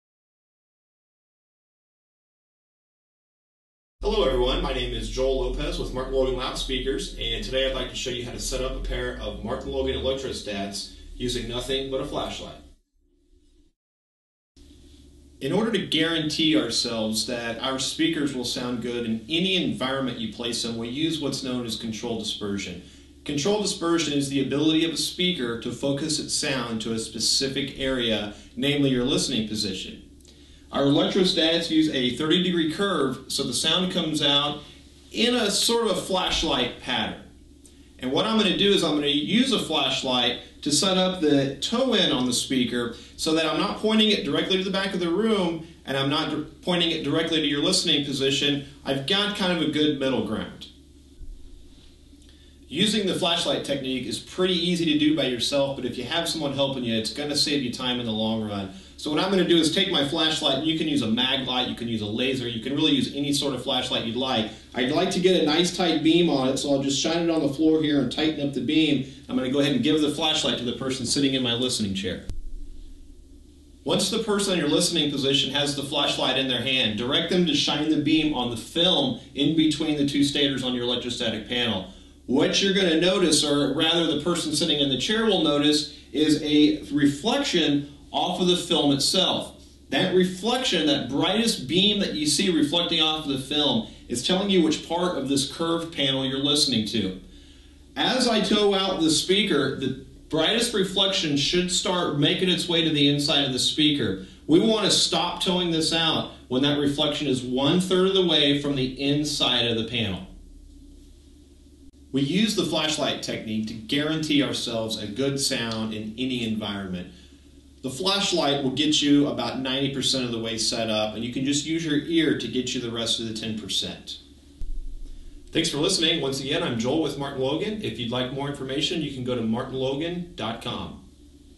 MartinLogan ESL Setup - The Flashlight Technique - 1080p HD
A short video explaining how to use a flashlight to properly setup a pair of electrostatic loudspeakers.

ESL speakers can be tricky to setup, but using this technique, you can easily get a good start to getting the best sound out of your speakers.